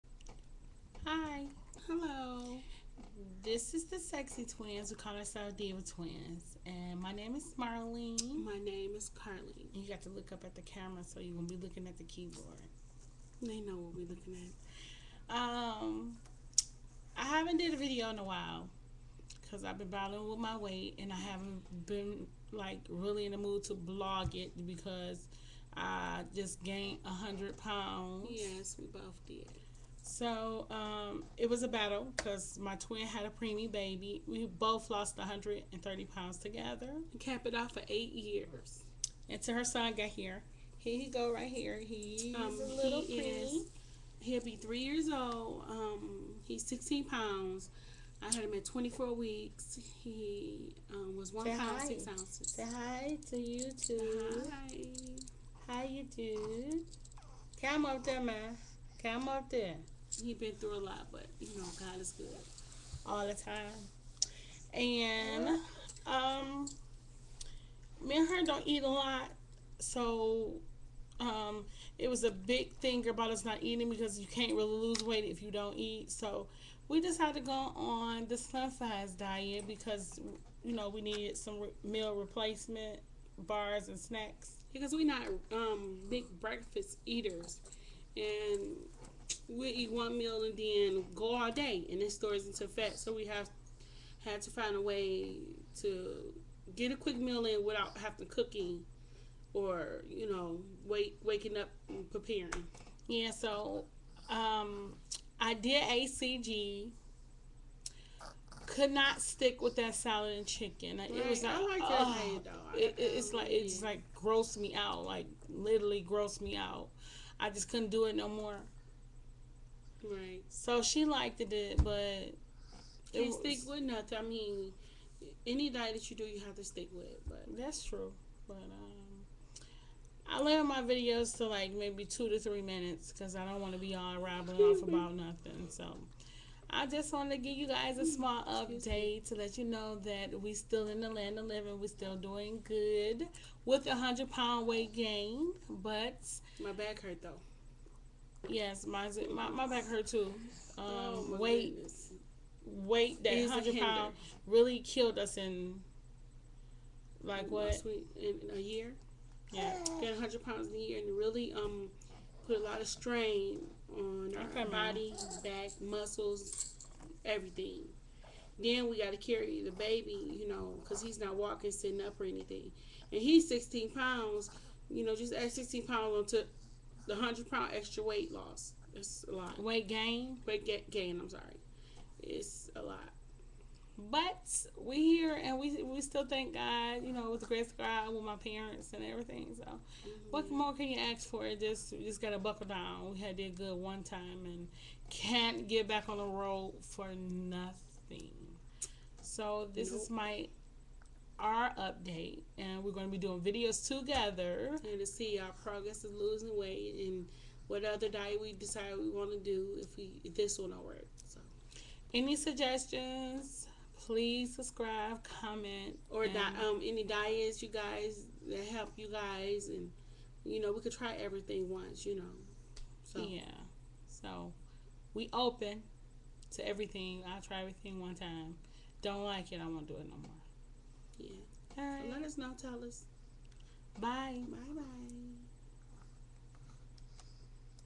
Diva twins update on big 100 pound weight gain wow
update on weight gain.. with my twin on slim fast!